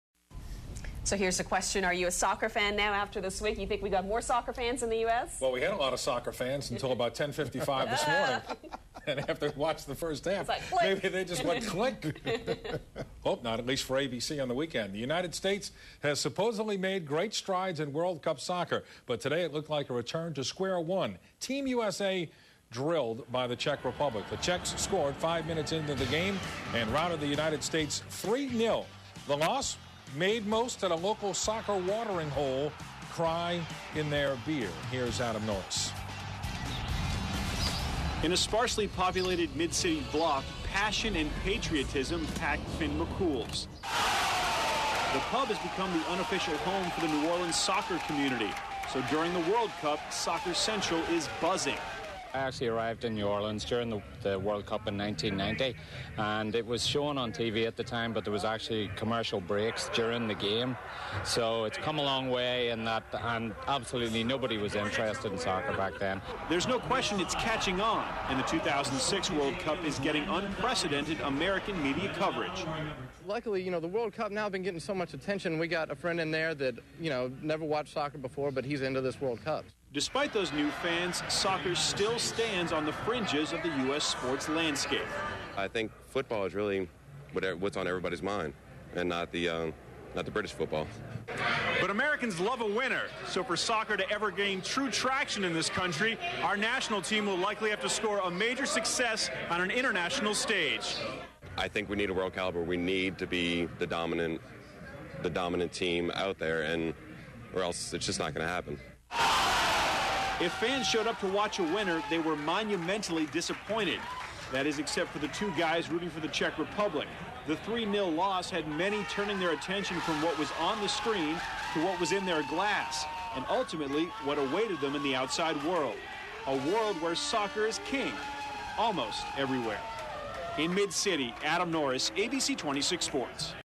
Finn McCool's World Cup 2006
A packed bar at Finn McCool's in New Orleans during WC2006. (WGNO)